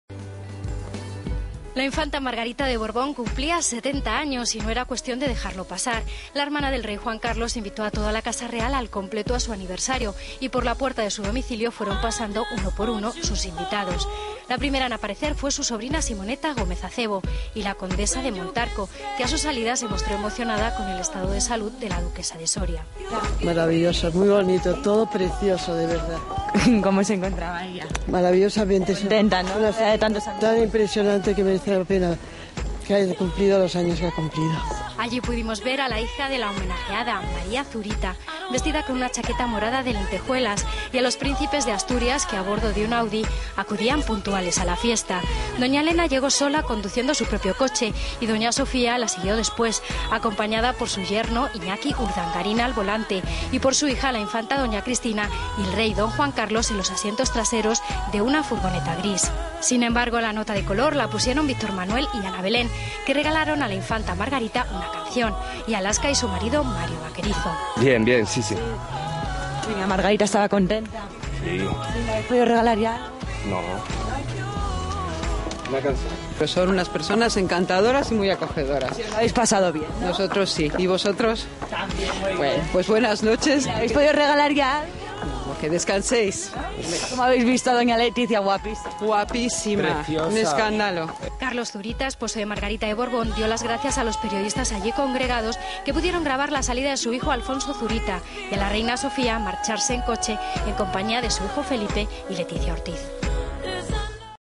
El 70 cumpleaños de la Infanta Margarita
Les hablamos de una celebración en la que estuvo toda la familia real. El motivo era el cumpleaños de la Infanta Margarita, que cumplia 70 años y su familia, al completo, quiso acompañarla. El rey, la reina, los príncipes, las infantas... Los Gómez Acebo y como no su marido y sus hijos, entre ellos María Zurita, que estaba guapísima de morado.